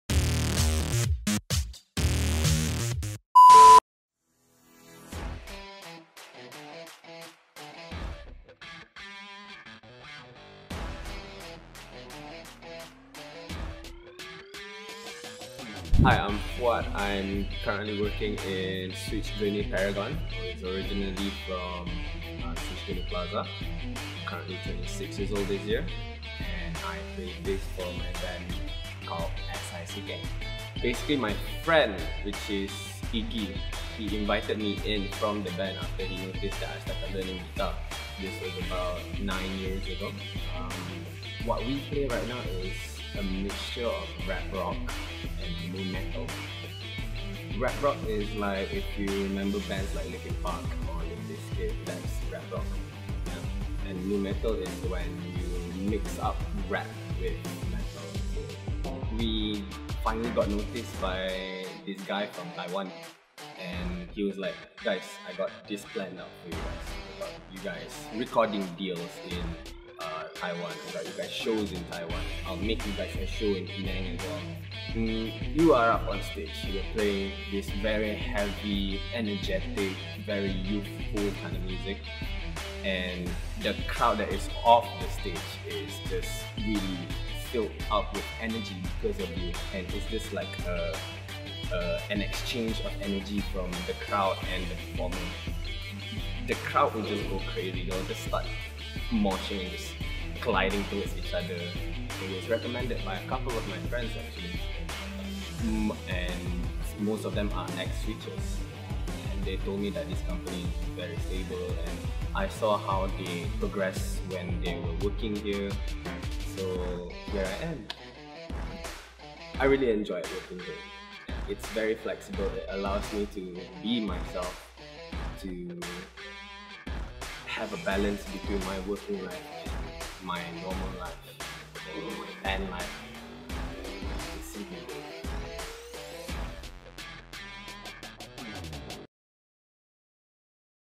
SwitchBuzz - FUAD
Fuad is all about music, this guy feels the vibes everyday, He has his own band called S.I.C.K. If you guys are interested to know more about the band click on the links given.